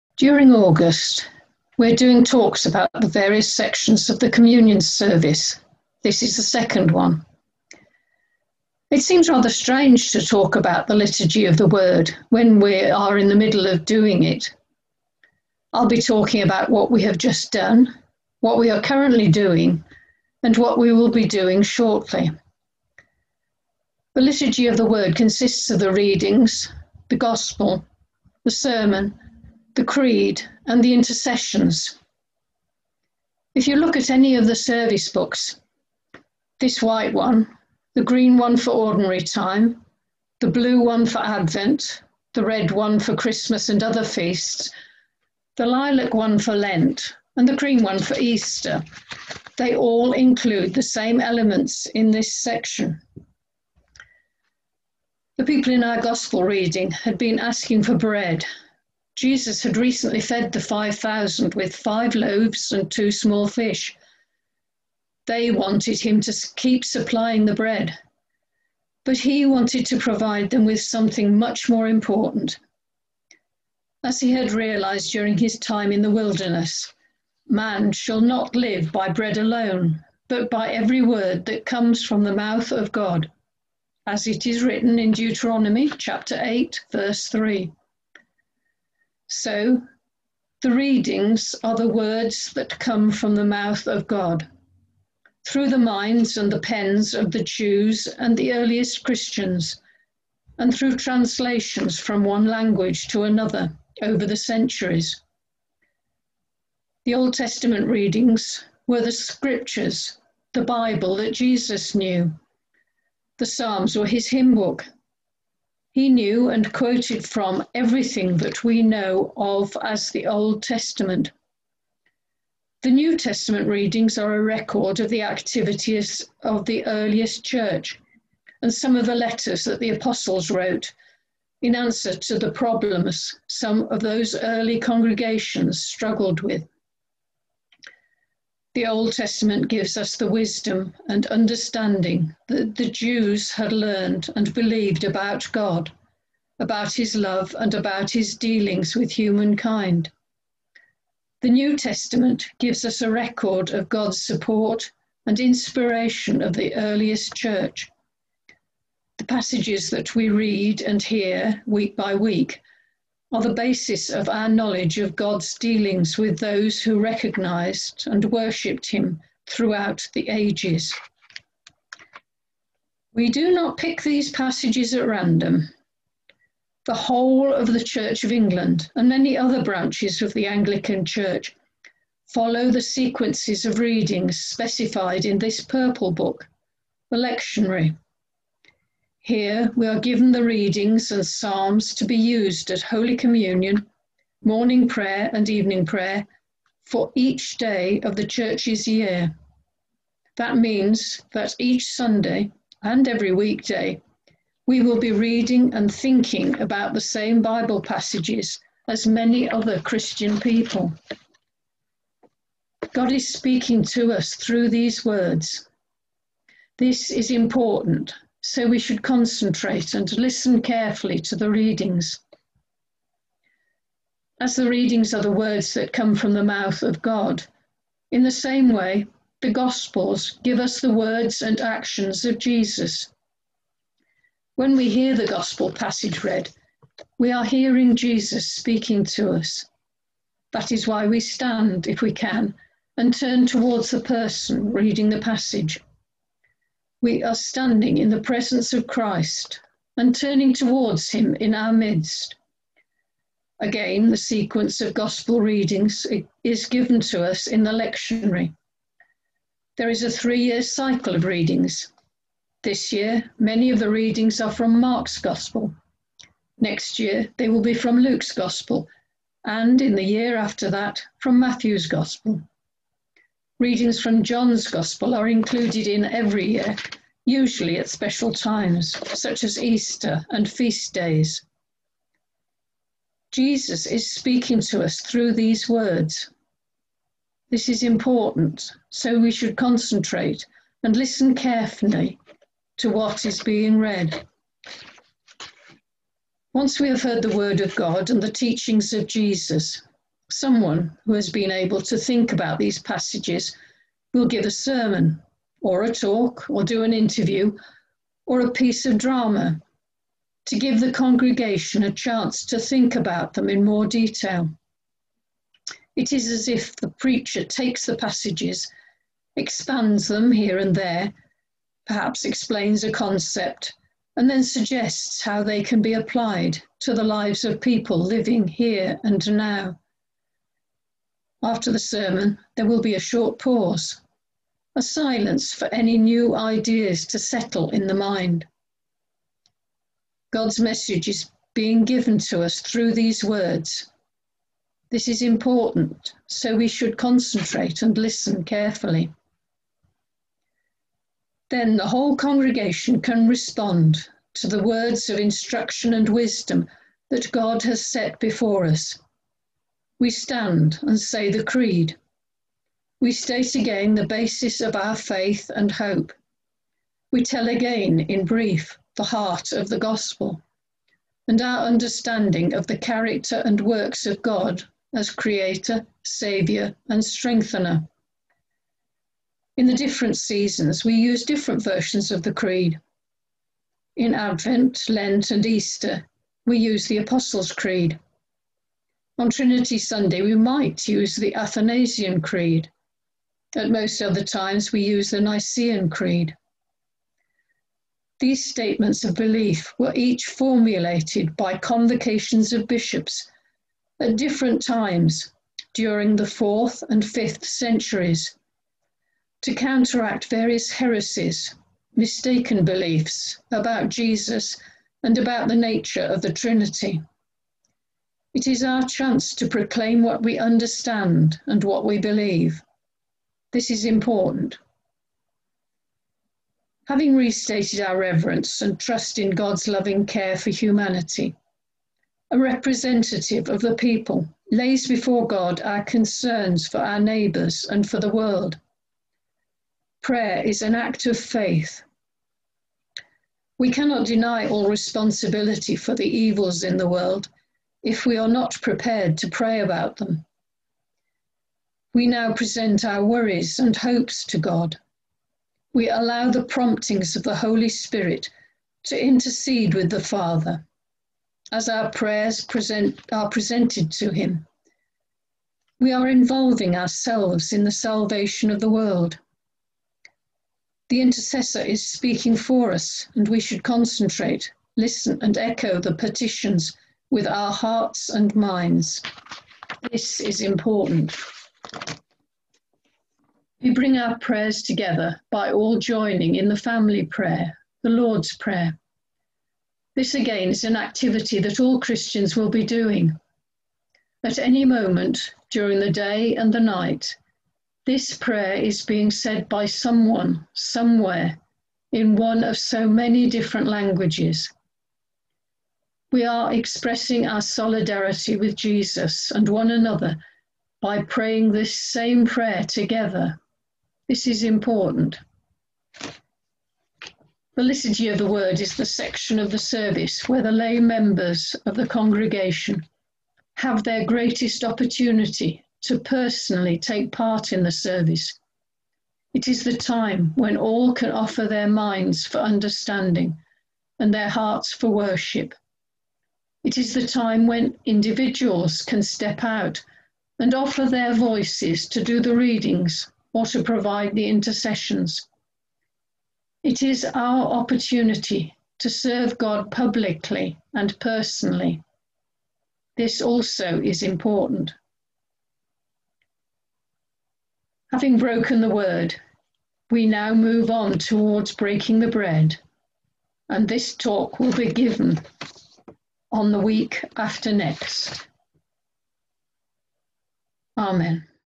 Sermon by our Reader, Janice Wones, for the Tenth Sunday of Trinity 2021 (8th August)
Please note that Janice's sermon was recorded after the service due to technical issues today...